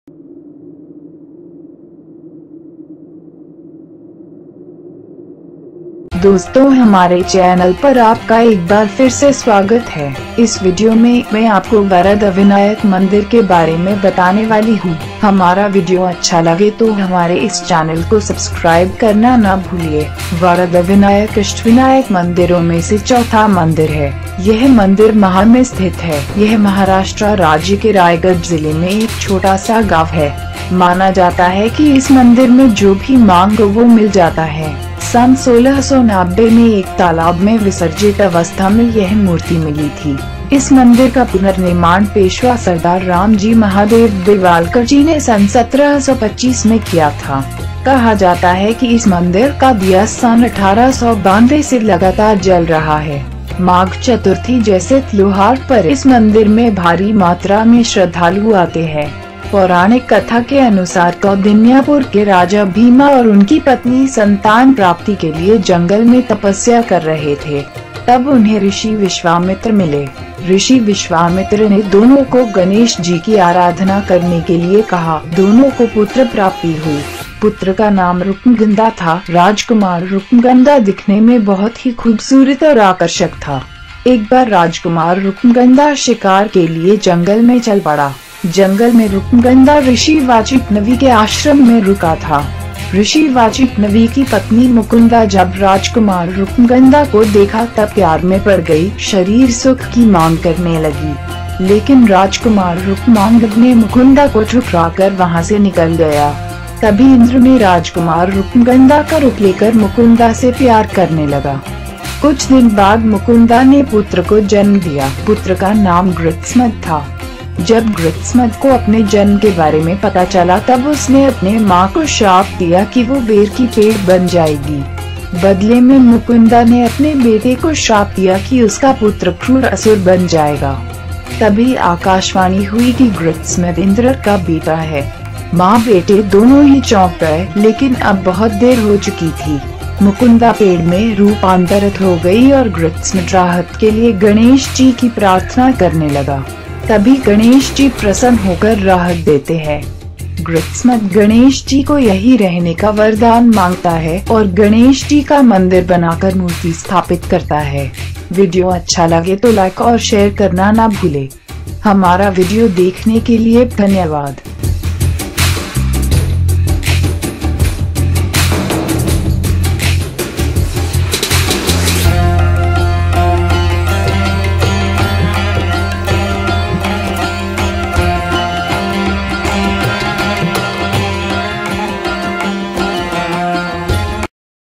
True Story of Varadvinayak Temple – Hindi
Varadvinayak temple is located in Mahad, Raigad, Maharashtra. This is one of the Ashtavinayak temples. And Varadvinayak temple is spiritual site in Maharashtra well as whole india. This is ganesh temple. In this video you can watch History of Varadvinayak Temple.